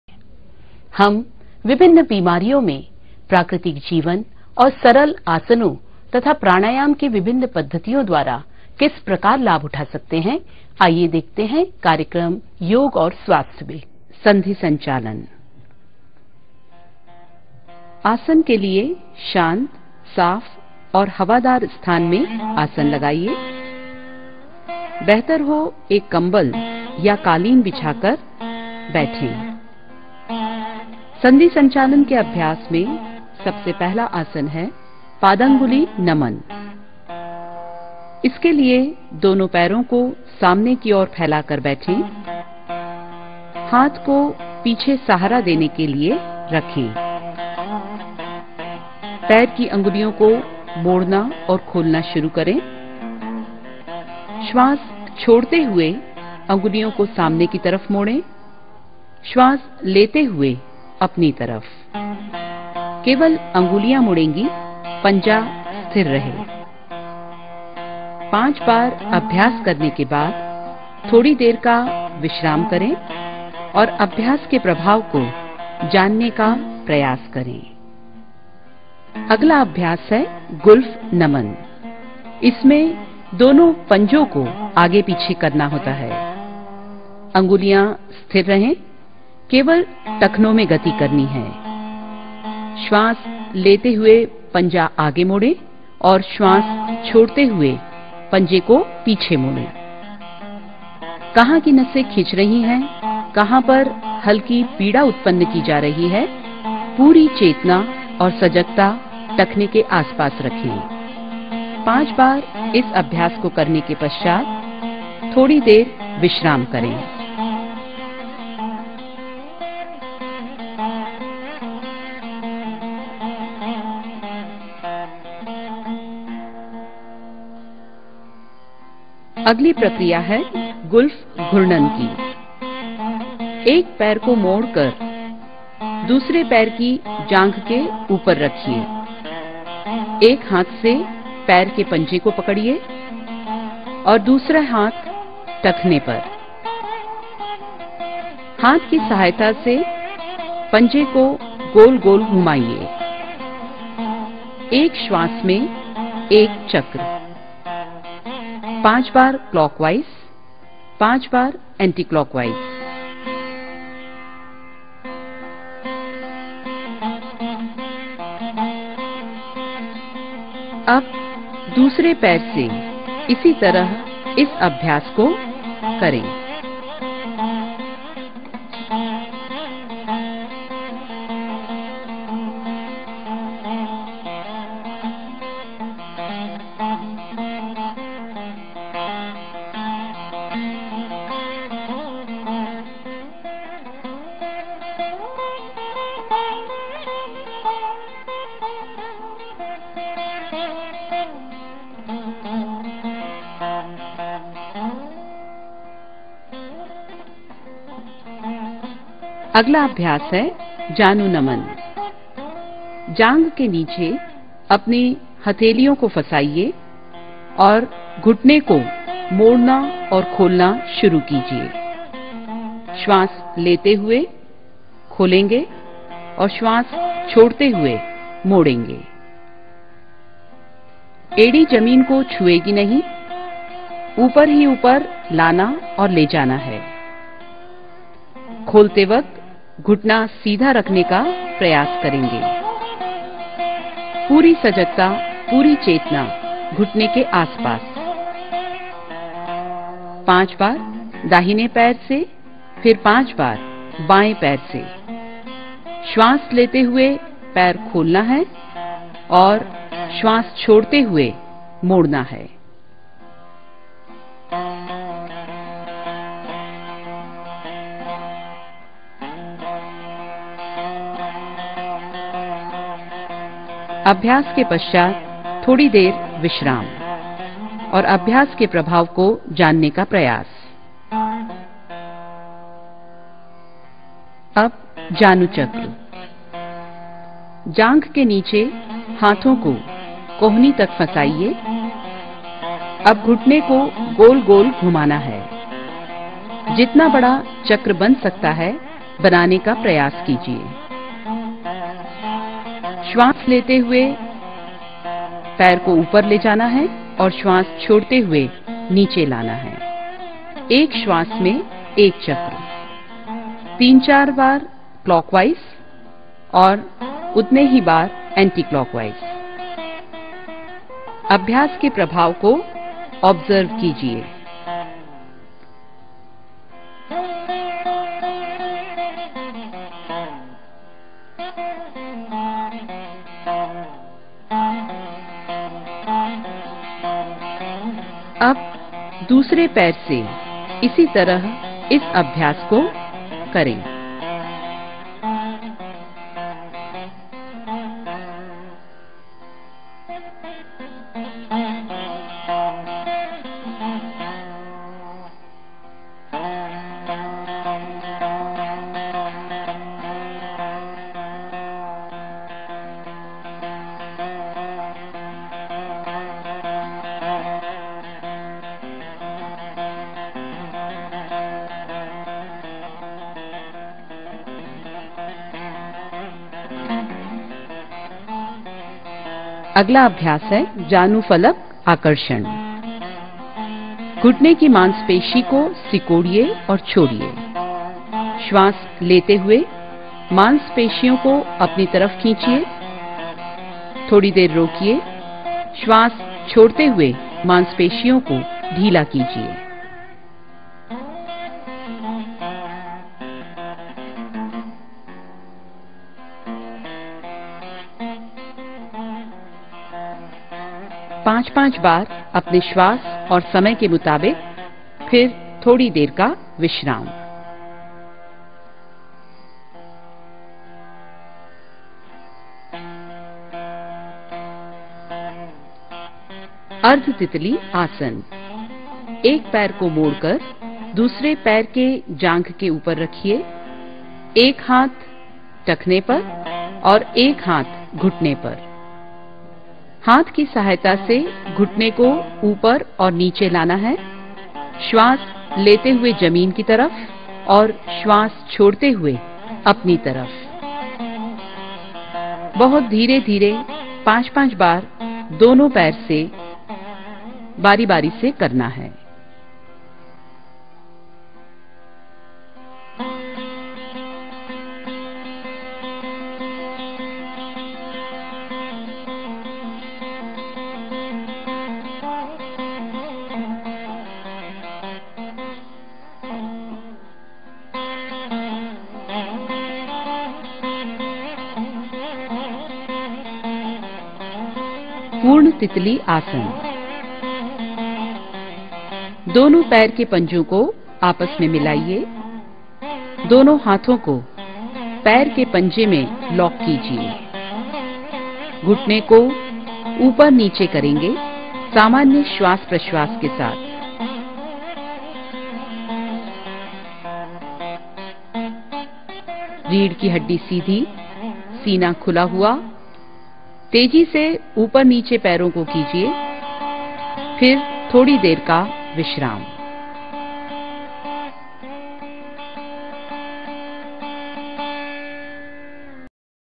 Sandhi Sanchalan-1.mpg
प्रात:काल खाली पेट संधि संचालन करने से संधियों के वात विकार दूर होते हैं ।